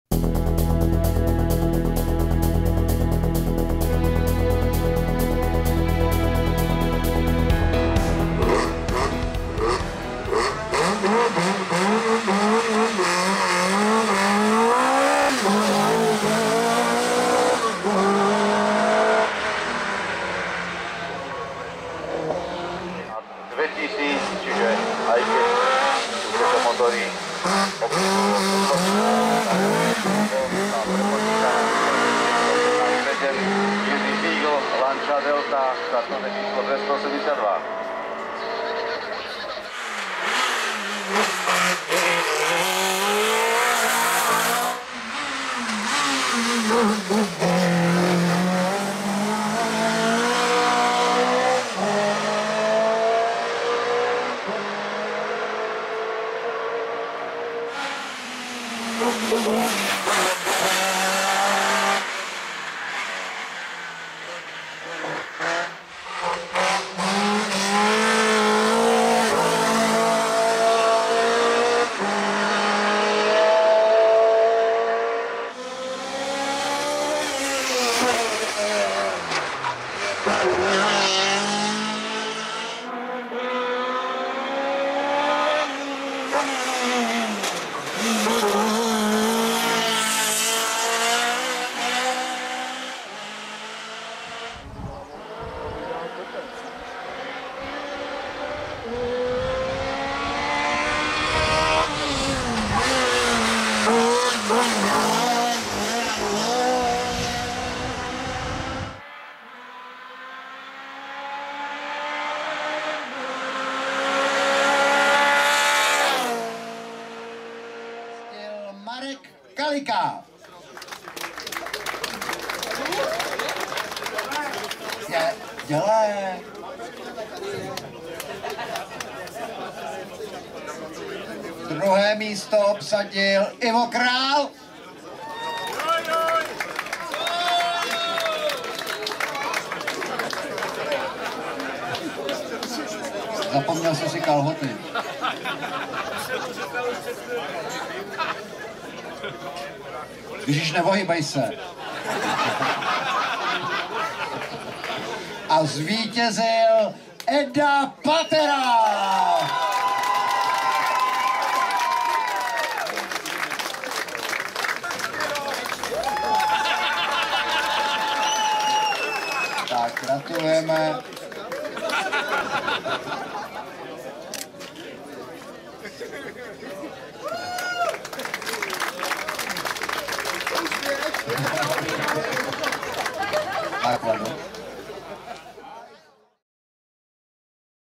VW Scirocco - Eda Patera - HOSTR Prix Liberec Ještěd 2017
Eda Patera si se svým rychlým VW Scirocco vyjel na kopci pod Ještědem 1.místo v obou víkendových závodech EDDA CUPu ve třídě H 2000.